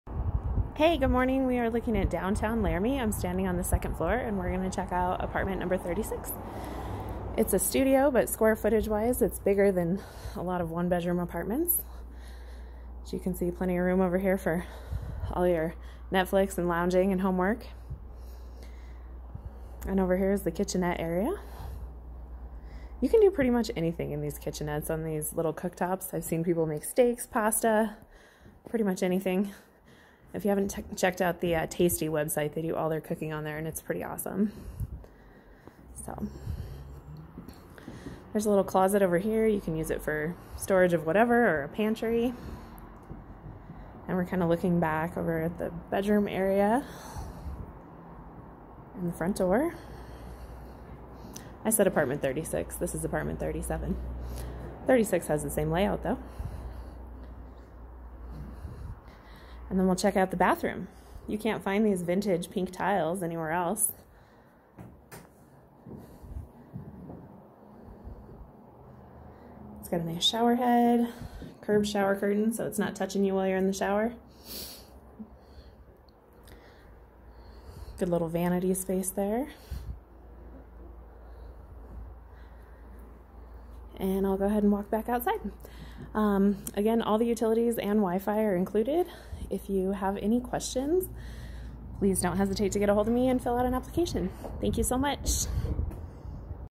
EXTRA LARGE STUDIO - BEST LOCATION: DOWNTOWN LARAMIE, 4 BLOCKS FROM UW
ALL UTILITIES, COURTESY FREE WIFI & RESERVED OFF-STREET PARKING INCLUDED W/Rent.